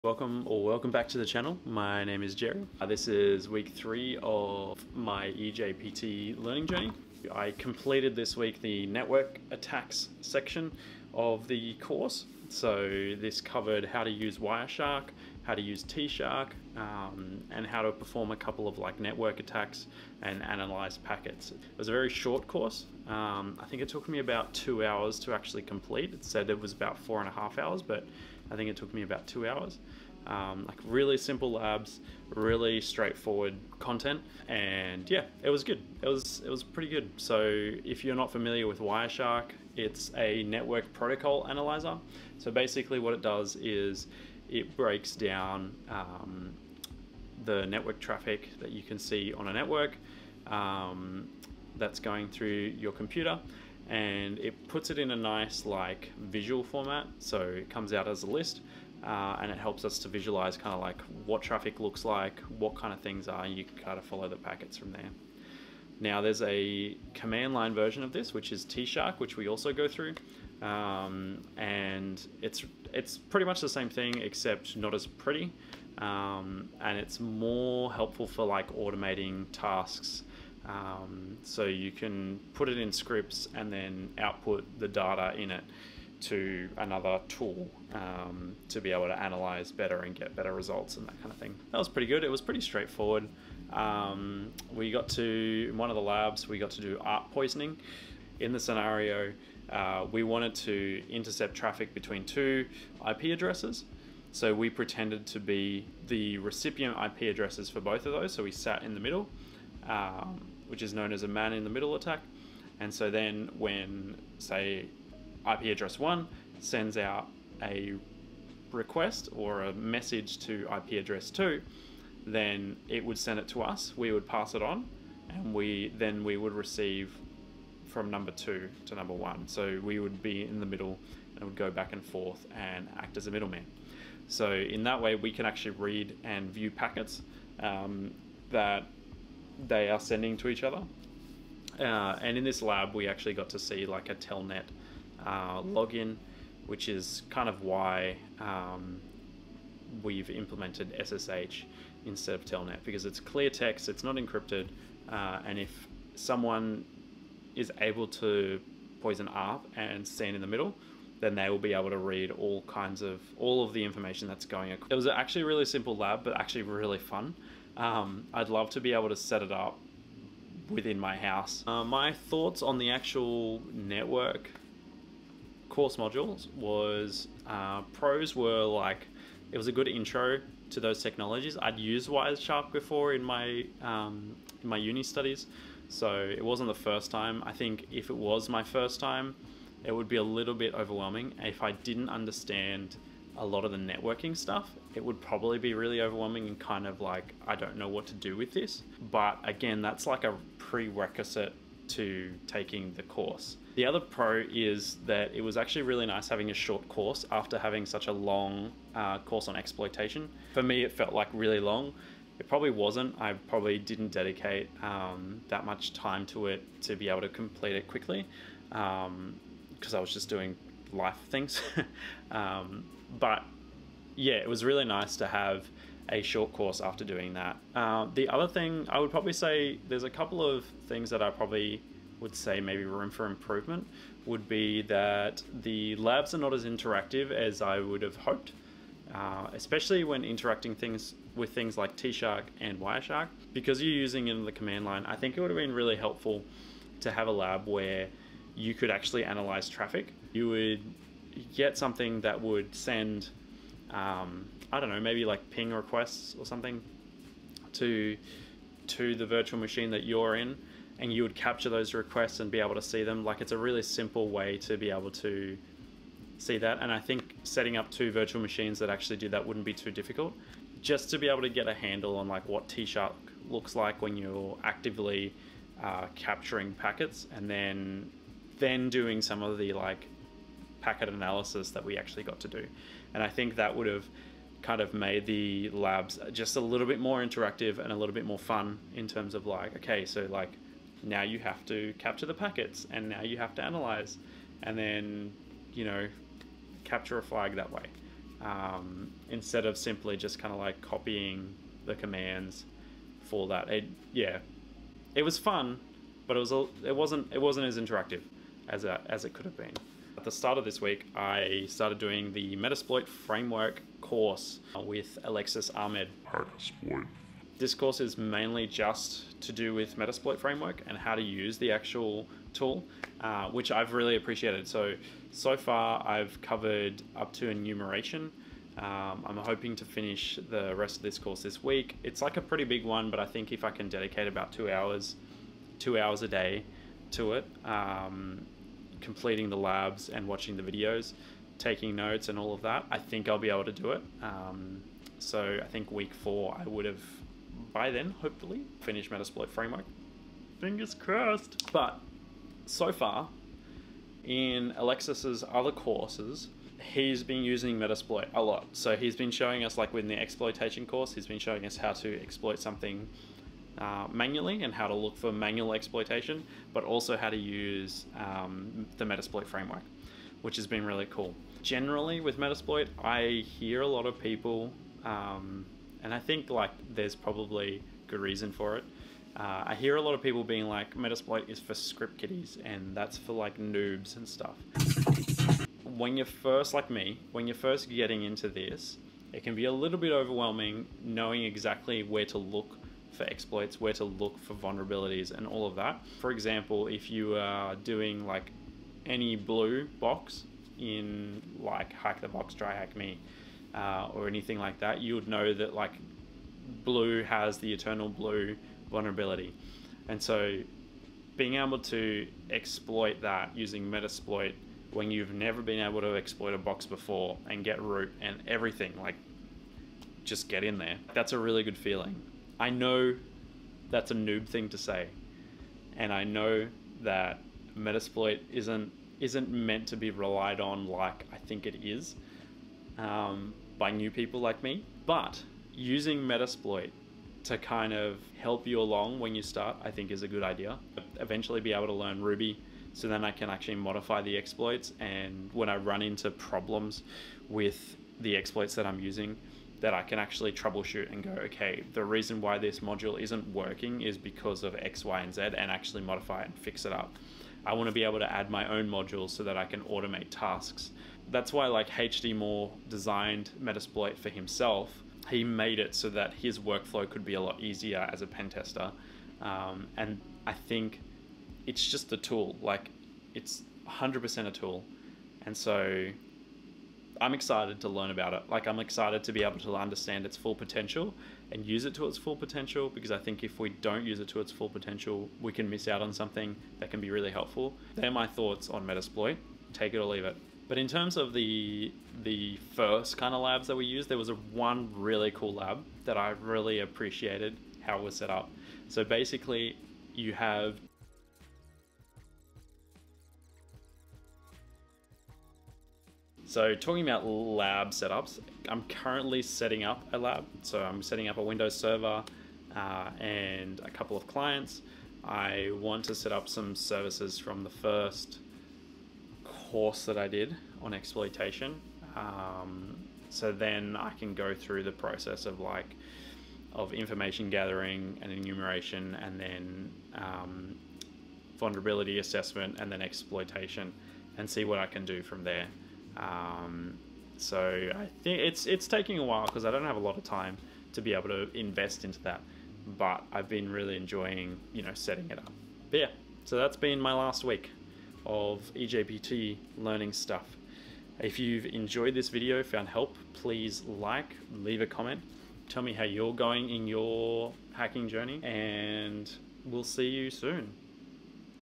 learning to HACK networks | Journey to eJPTv2 | Week 3
This week I looked at hacking networks using Wireshark and Tshark. Went through basic usage and a great lab on ARP poisoning.
I also give my HOT TAKE on using Metasploit to learn to hack.

Hope this inspires you to keep going!